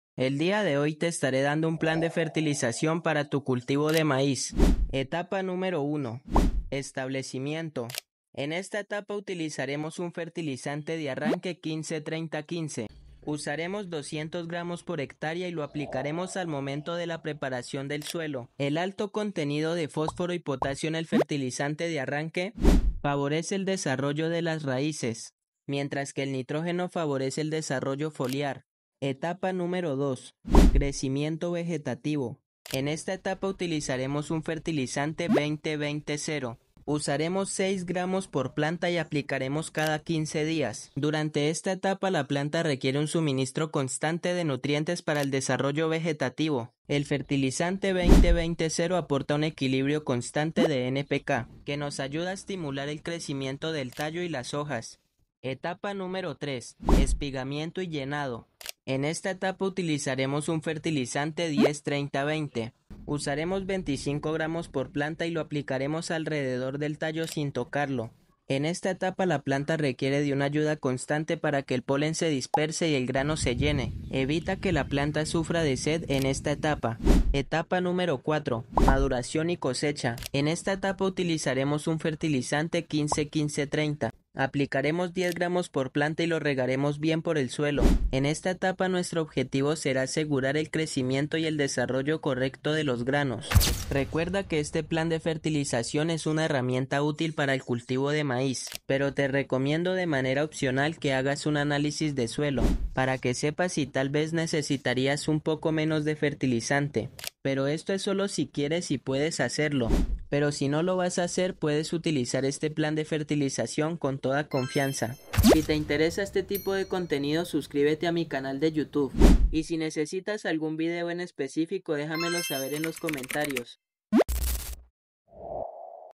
Plan de fertilización completo para maíz: ¡Maximiza tu cosecha!
¿Quieres obtener cosechas de maíz abundantes y de alta calidad? En este video te presentamos un plan de fertilización completo y efectivo para cada etapa del cultivo, desde el establecimiento hasta la cosecha.

Etapa 1: Establecimiento (0-1 mes)

Fertilizante: Arranque 15-30-15
Dosis: 200 g/ha
Aplicación: Mezclar con la tierra durante la preparación del suelo.
Beneficios:
Favorece el desarrollo de las raíces.
Promueve el crecimiento foliar.

Etapa 2: Crecimiento vegetativo (1-2 meses)

Fertilizante: 20-20-0
Dosis: 6 g/planta
Frecuencia: Cada 15 días
Beneficios:
Aporta un equilibrio de NPK.
Estimula el crecimiento del tallo y las hojas.

Etapa 3: Espigamiento y llenado (2-3 meses)

Fertilizante: 10-30-20
Dosis: 25 g/planta
Aplicación: Rodeando la planta.
Beneficios:
Favorece la polinización y el llenado del grano.
Evita el estrés de la planta.

Etapa 4: Maduración y cosecha (3-4 meses)

Fertilizante: 15-15-30
Dosis: 10 g/planta
Aplicación: Riego al suelo.
Beneficios:
Asegura el desarrollo del tamaño y la calidad del grano.

Consejos adicionales:

Realiza un análisis de suelo para ajustar la dosis de fertilizante según las necesidades específicas de tu terreno.
Sigue las instrucciones del fabricante del fertilizante.
Combina la fertilización con otras prácticas agrícolas como el riego adecuado, el control de plagas y enfermedades, y la rotación de cultivos